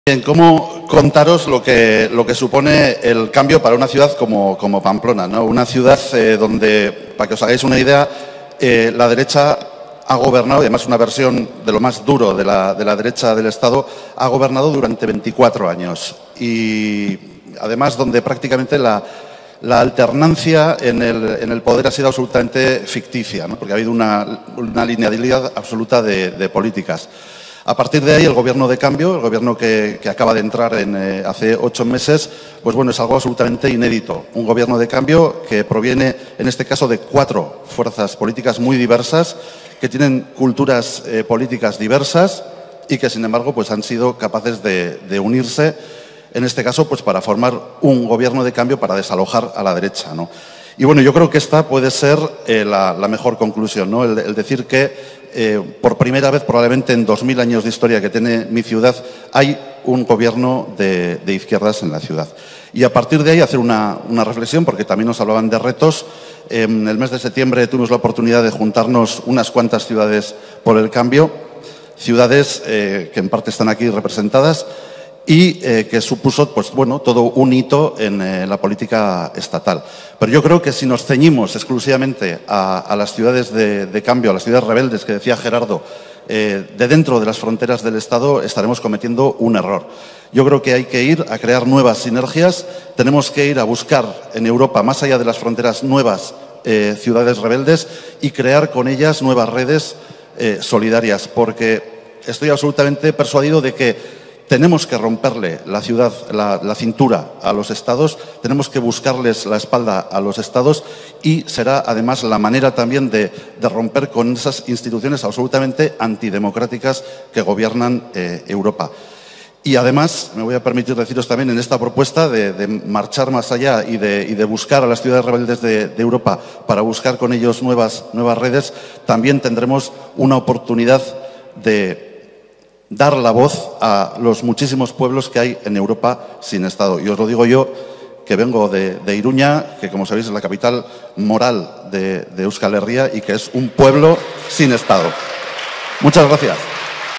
[Jornadas PlanB] Joseba Asiron propone crear redes solidarias entre ciudades rebeldes
Intervención del alcalde de Iruñea Joseba Asiron en las jornadas sobre el PlanB para Europa celebradas en Madrid.